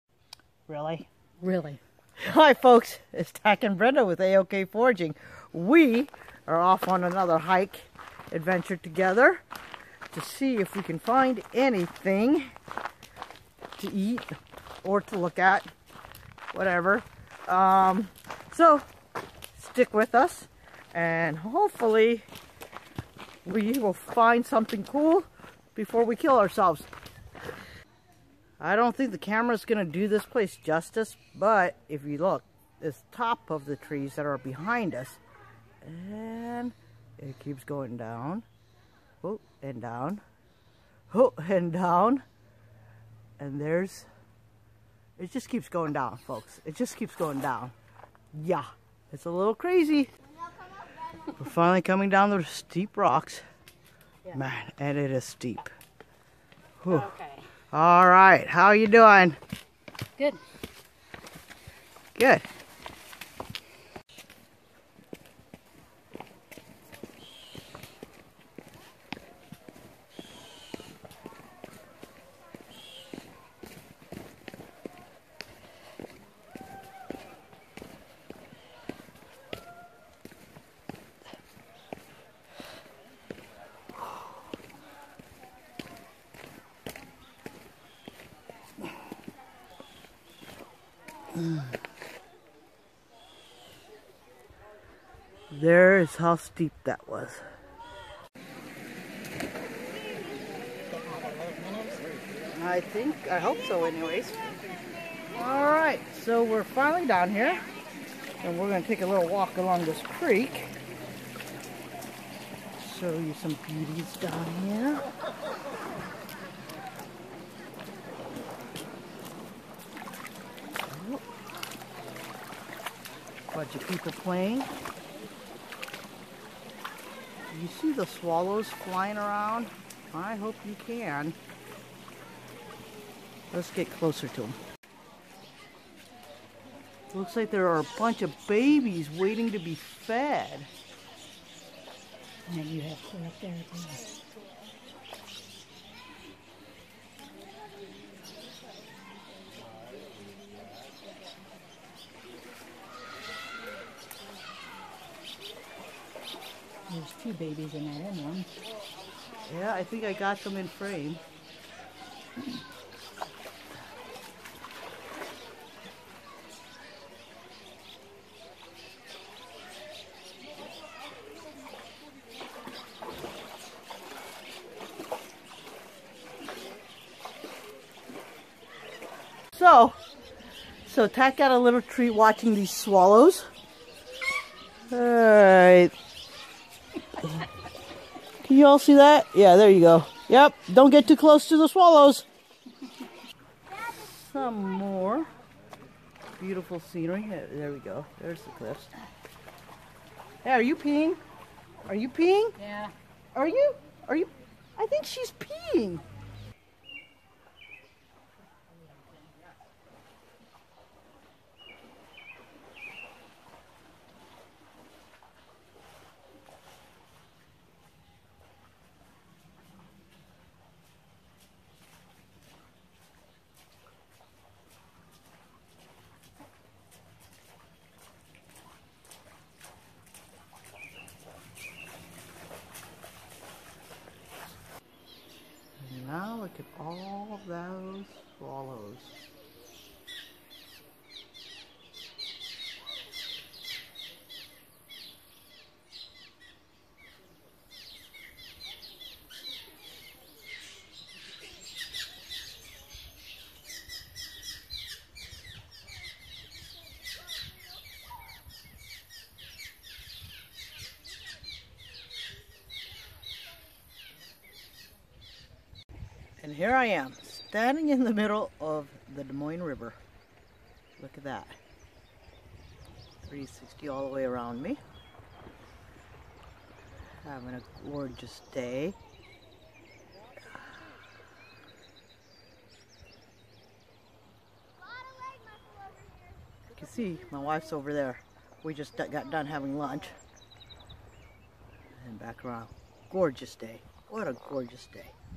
AOK Foraging and Adventures -7/8/17 Hike
Day hike at Ledges State Park. Missing pieces of the wild edibles I found and the ending of the video so it came to an abrupt end.